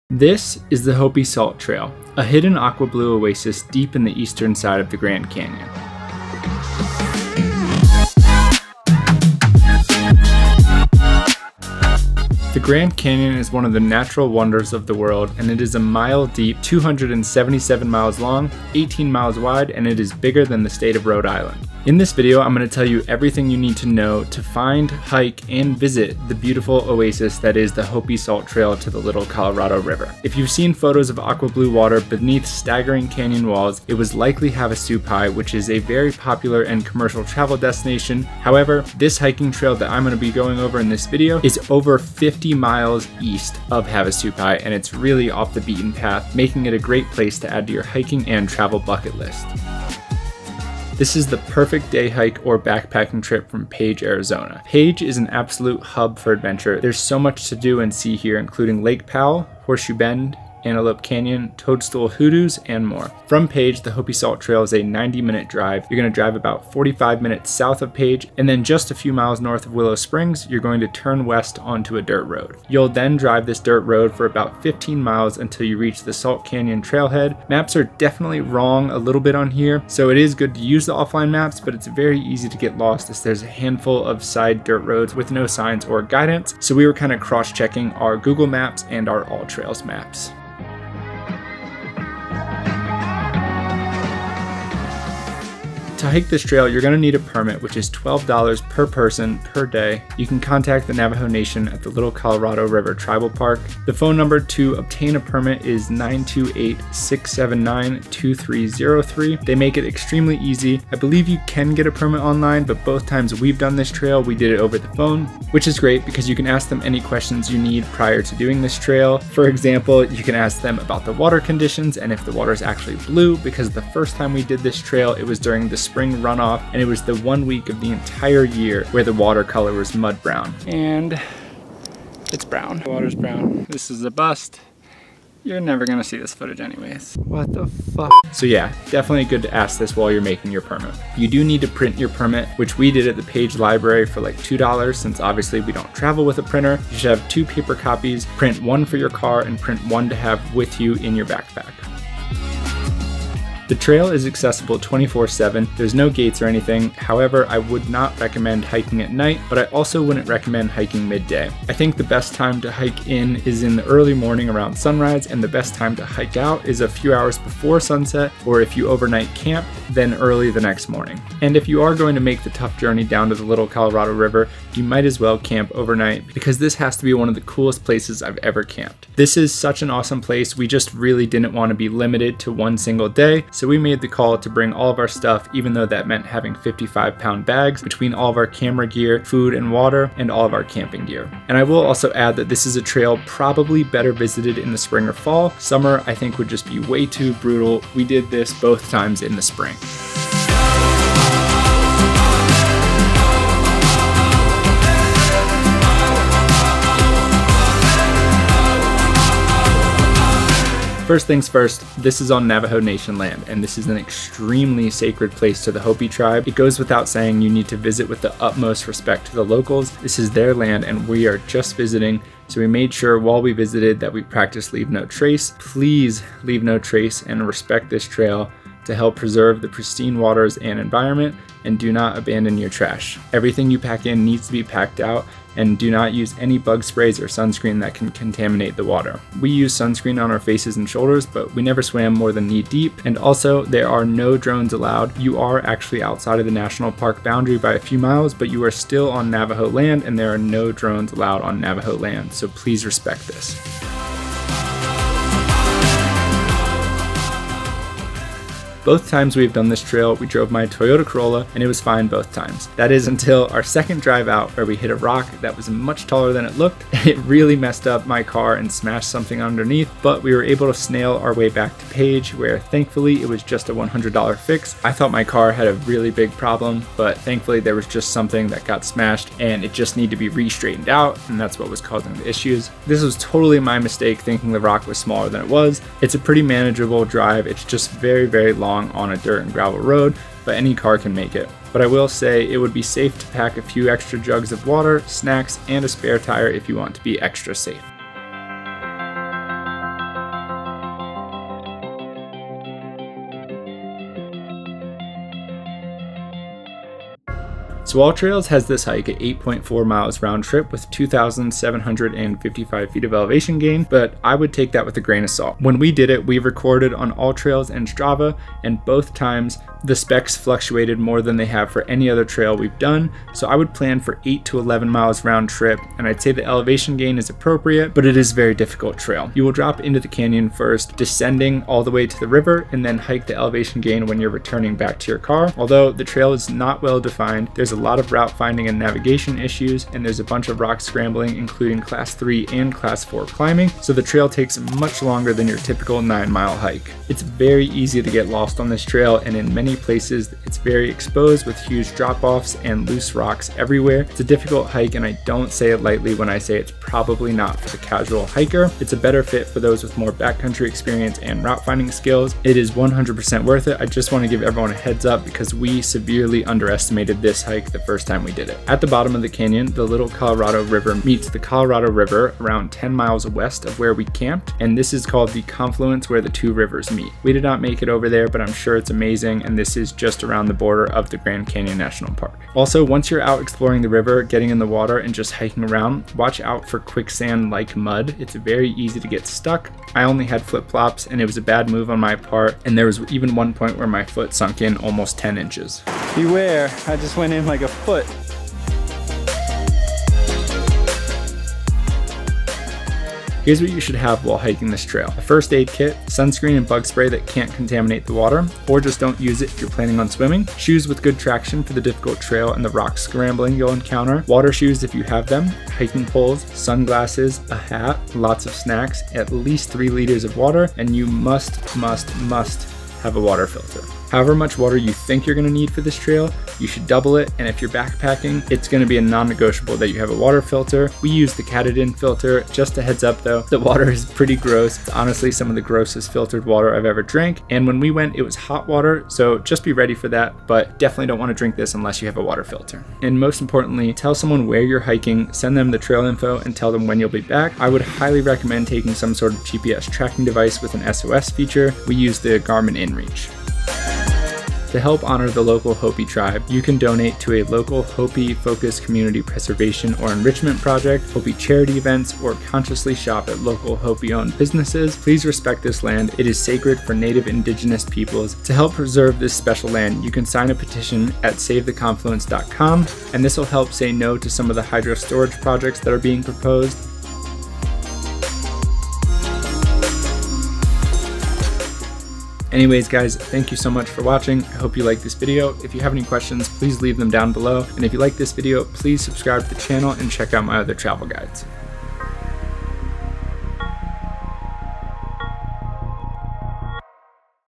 How To Visit The Grand Canyon's Aqua Blue Oasis - Arizona Hidden Gem
Everything you need to know to hike the Hopi Salt Trail to The Little Colorado River. This is the most beautiful place I've hiked to in the entire country, definitely worth adding to your travel bucket list!

Created by: Kevin Eassa Media
Featuring: Bri Amato @breezydaysahead , Hayden Lynch @haydlynch

My Equipment: